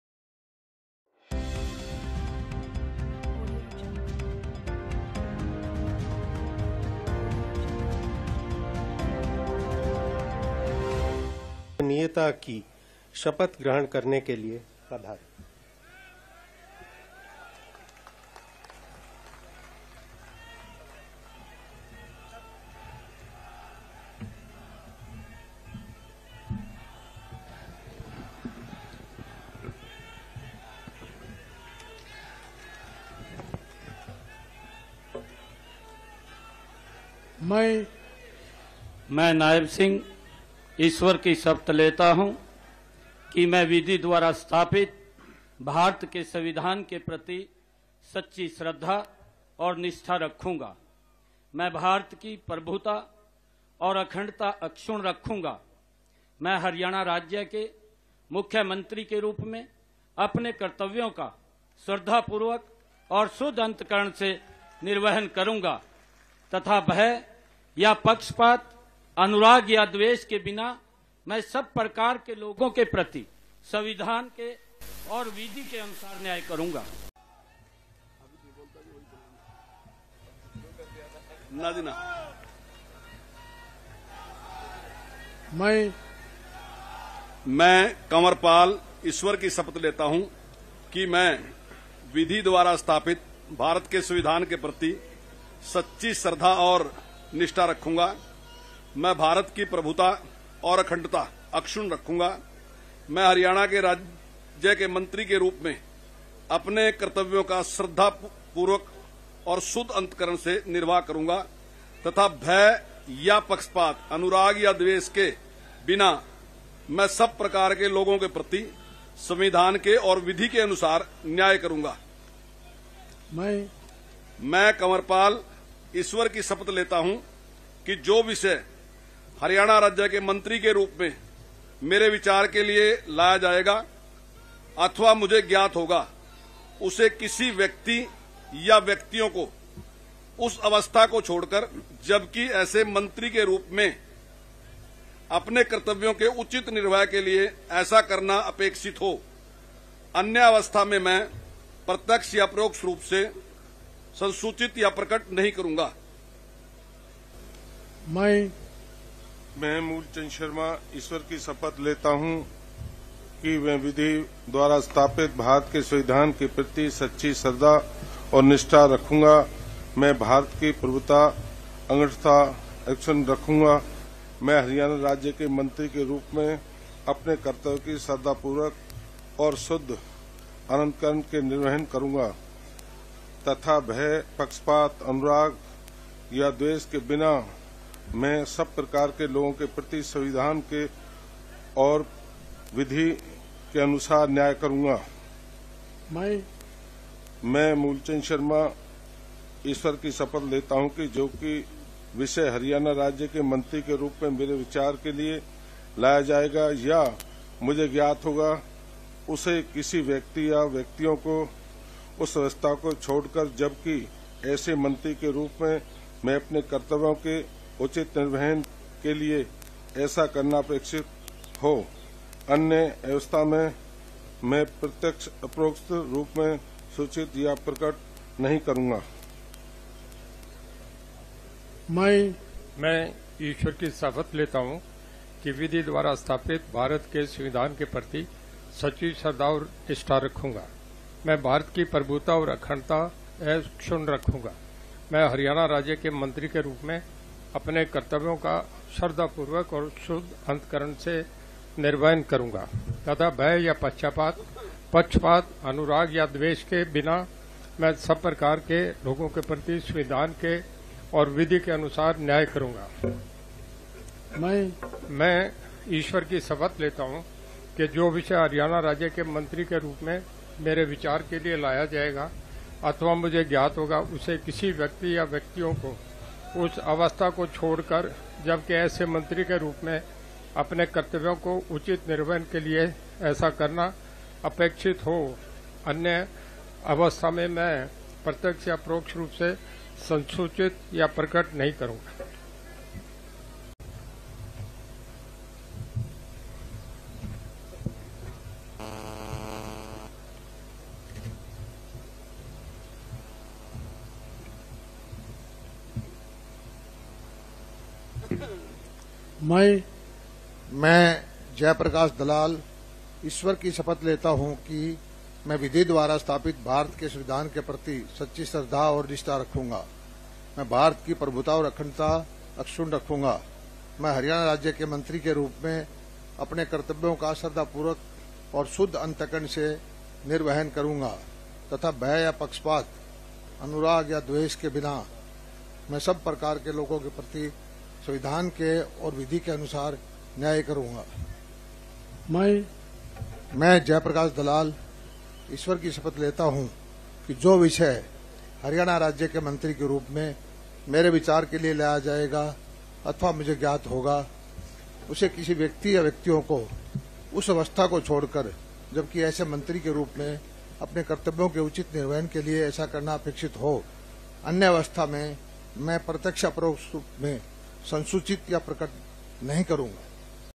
Haryana के नए CM बने Nayab Singh Saini, एक निर्दलीय समेत 5 विधायक बने Minister
Haryana में नायब सैनी की ताजपोशी हो गई है। राज्यपाल बंडारू दत्तात्रेय ने राजभवन में आयोजित एक कार्यक्रम में उन्हें पद और गोपनीयता की शपथ दिलाई।